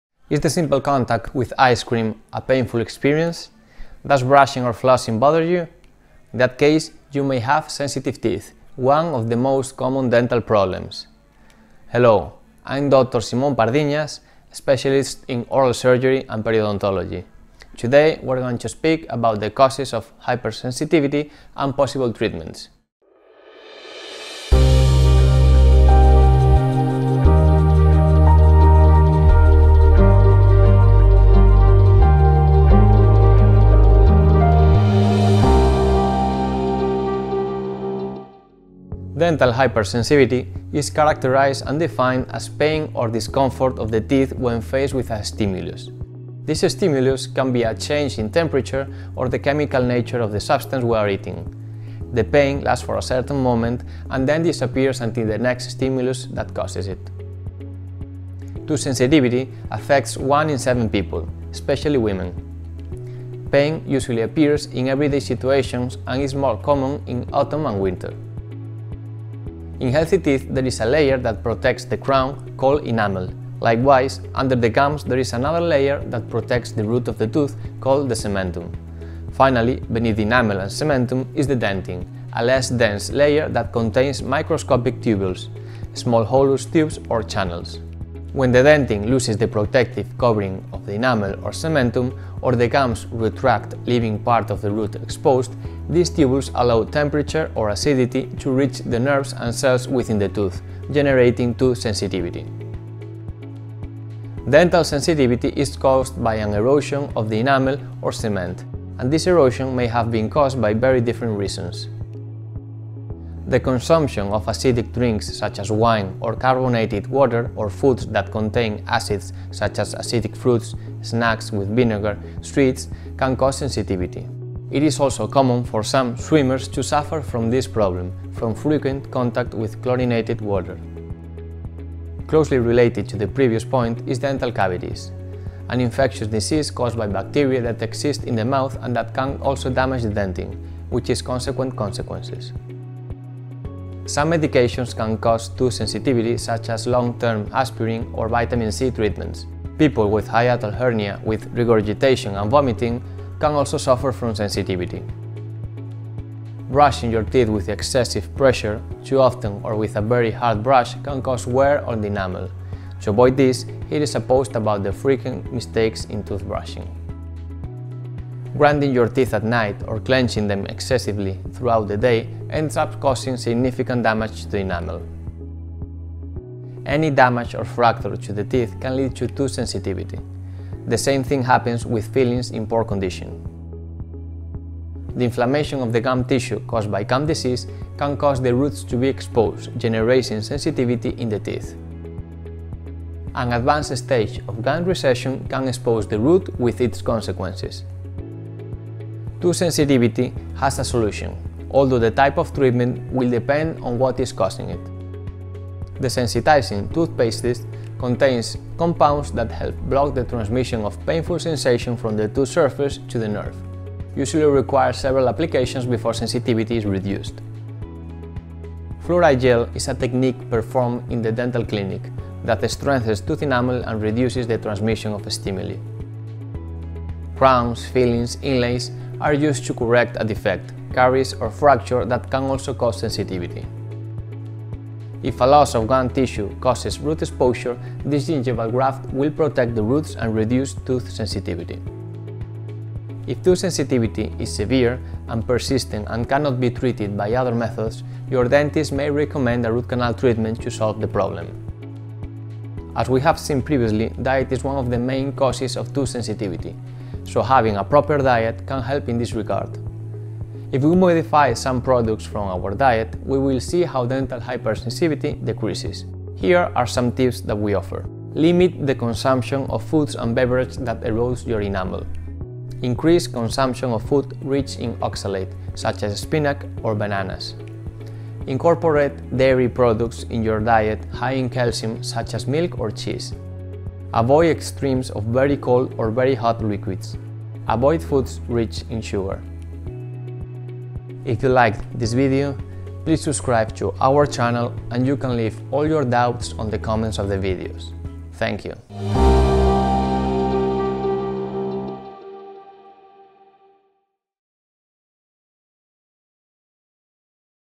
Sensitive tooth? - Treatment of DENTIN HYPERSENSITIVITY and its causes | Dentalk! ©
Is the simple contact with ice cream a painful experience? Does brushing or flossing bother you? In that case, you may have sensitive teeth, one of the most common dental problems.

1) Introduction: 00:00.
In this Dentalk from Clínica Médico Dental Pardiñas we talk about the causes of dentin hypersensitivity and possible treatments.

2) What is tooth sensitivity? 00:39.
Tooth sensitivity is pain or discomfort of the teeth when faced with a stimulus. This stimulus can be a change in temperature or the chemical nature of the substance we are eating.

3) What is the origin of sensitive teeth? 01:10.
When the dentin loses the protective covering of the enamel or the gums retract, temperature or acidity reach the nerves and cells within the tooth, generating tooth sensitivity.

4) Causes of dentinal hypersensitivity: 01:45.
Dental sensitivity is caused by an erosion of the enamel or cement.And this erosion may have been caused by very different reasons such as diet, tooth decay, medication, improper brushing, bruxism, dental fractures or wear, periodontal disease or recession of the gums.

5) Treatment of sensitive tooth: 03:20.
Tooth sensitivity has a solution, although the type of treatment will depend on what is causing it. Among the common options are desensitizing toothpastes, fluoride gel, dental crowns, fillings or inlays, gum grafting or endodontics in more severe cases.

6) Diets that help prevent dentin hypersensitivity: 04:13.
Eating a proper diet can help in this regard. It is recommended to avoid sugary or very cold / hot drinks and foods and increase the consumption of oxalate and dairy products.

Dentalk is a project of the Pardiñas Dental Medical Clinic in which we answer your questions about oral health. More Dentalk videos